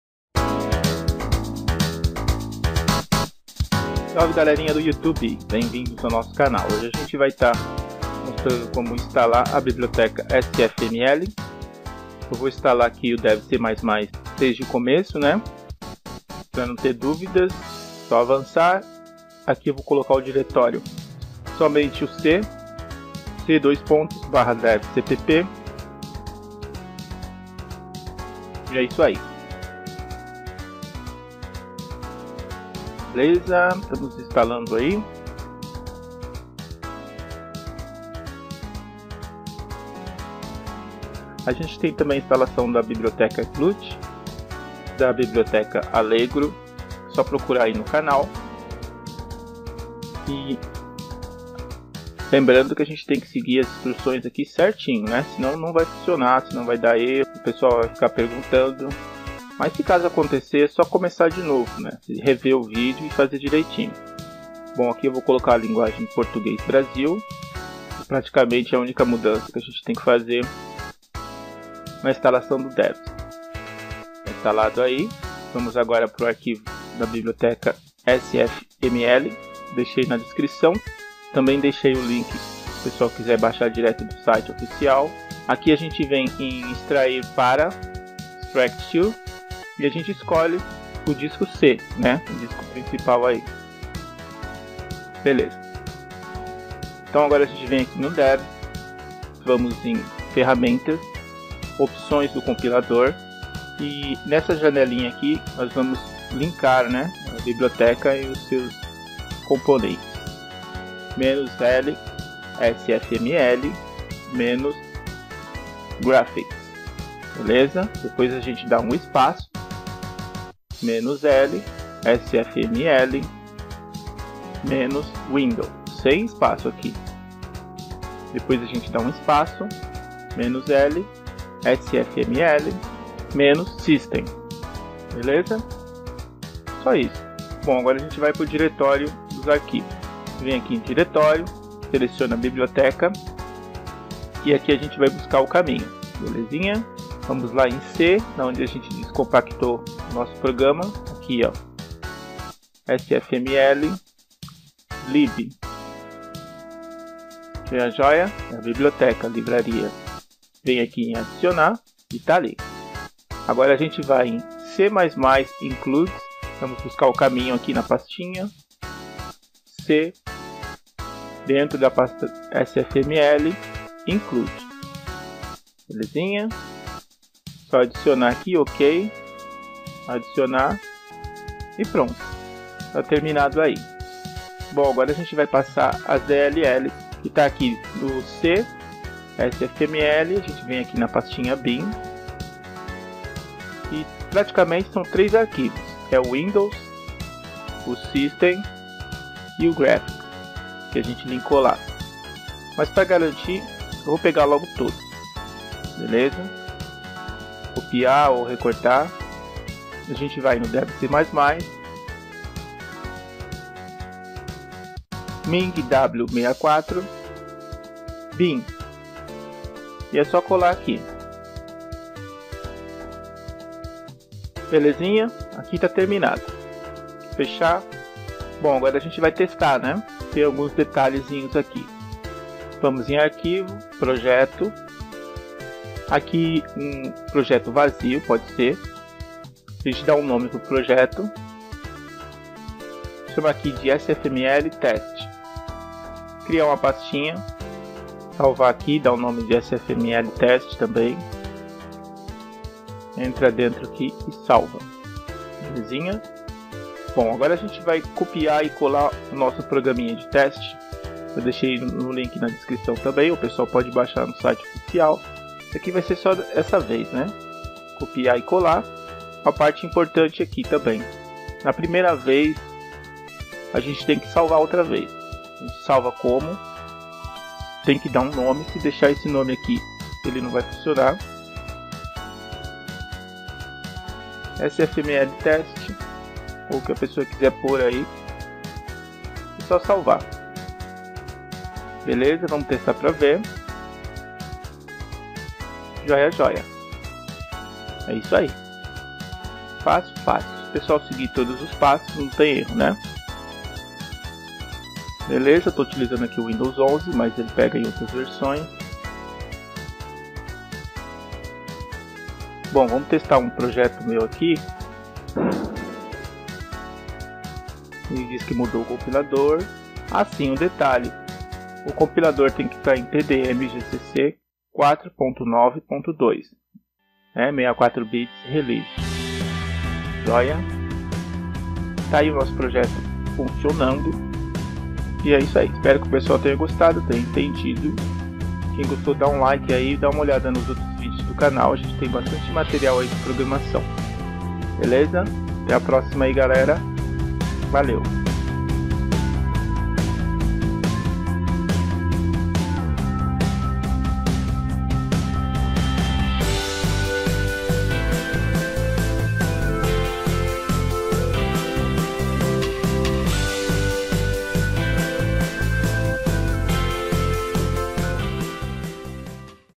Download and install SFML Devc 5 11 tutorial
Baixar e instalar SFML Devc++ 5.11 tutorial

-lsfml-graphics
-lsfml-window
-lsfml-system
confira este vídeo com o @DanielOliveira00